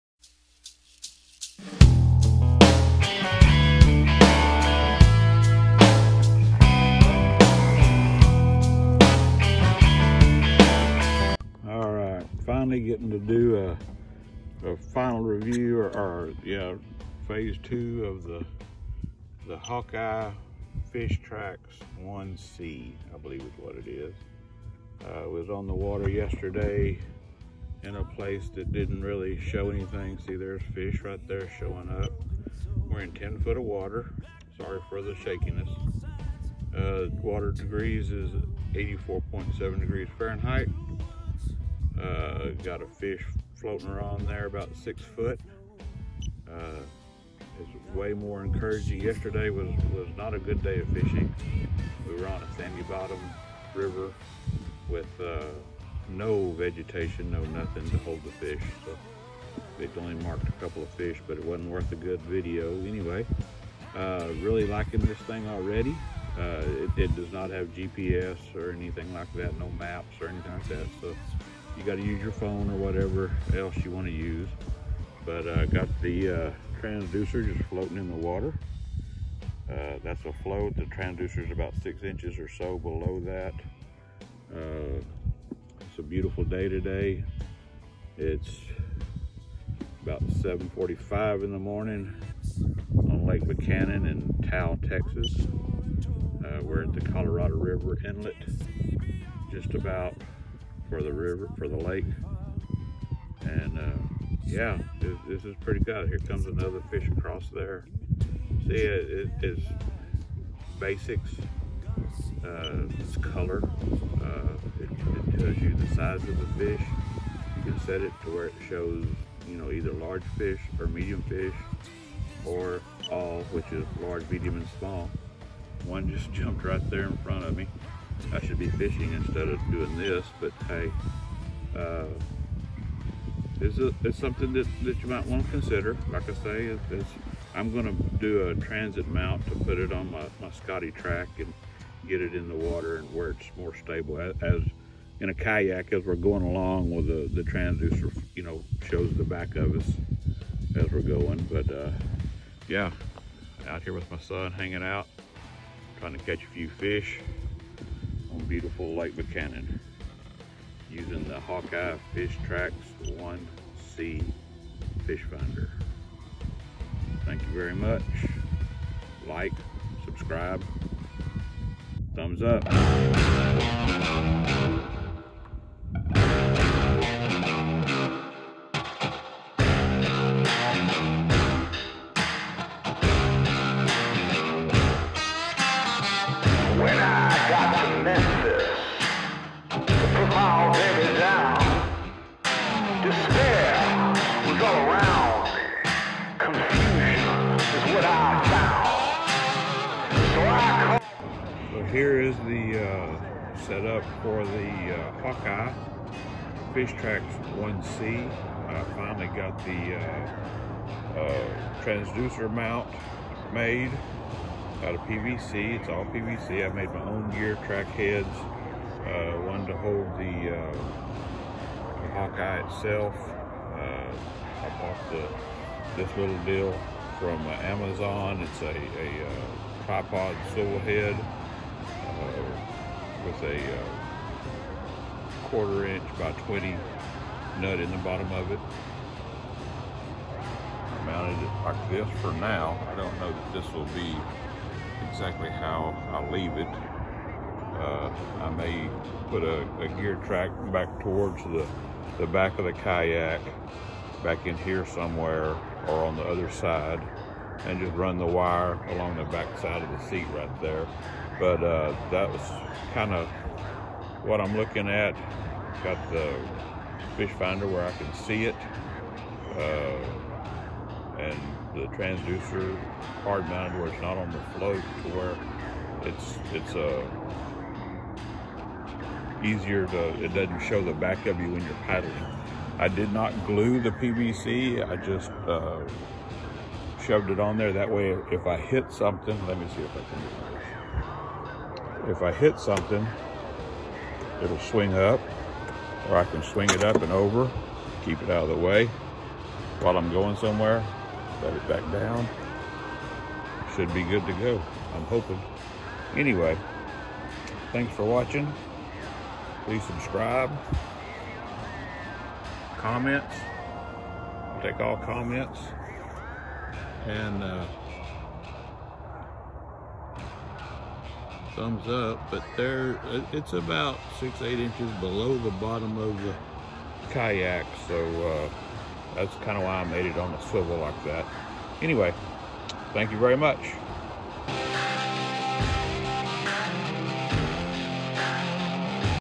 HawkEye FishTrax C1 fish finder part 2 - On the Water
Reviewing the HawkEye FishTrax C1 fish finder while on the water.  Also building the transducer mount for the kayak

Kayak - Pelican Catch 120
Video - iPhone XR  - Sorry for the poor quality still learning how to edit videos.